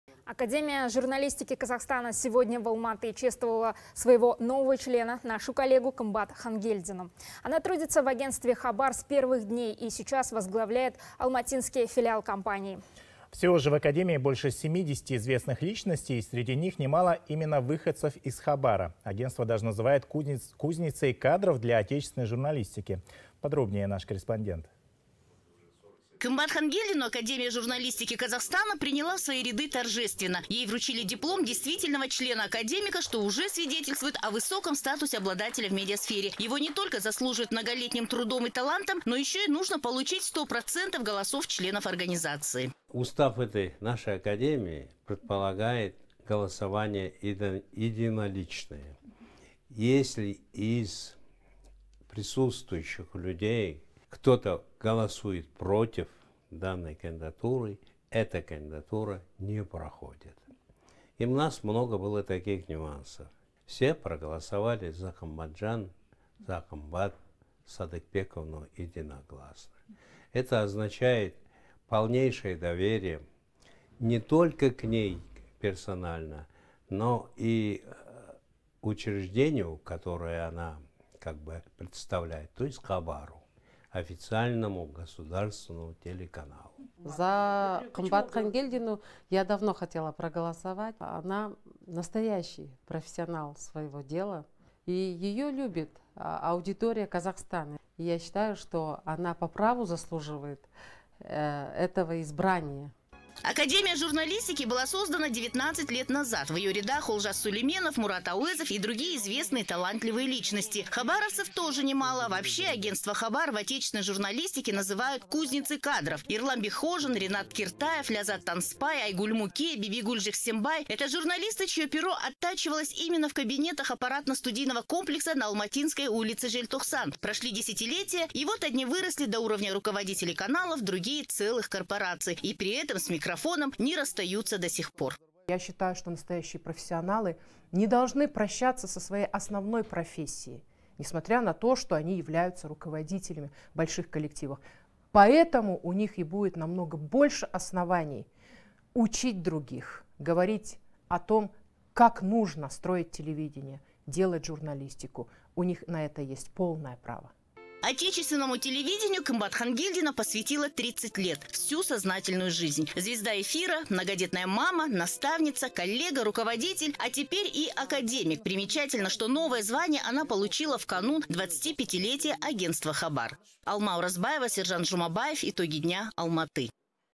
В Алматы прошло пополнение в рядах Академии Журналистики Казахстана
Академия журналистики Казахстана сегодня в Алматы чествовала своего нового члена – нашу коллегу Кымбат Хангельдину.
Она трудится в Агентстве «Хабар» с первых дней и сейчас возглавляет Алматинский филиал компании.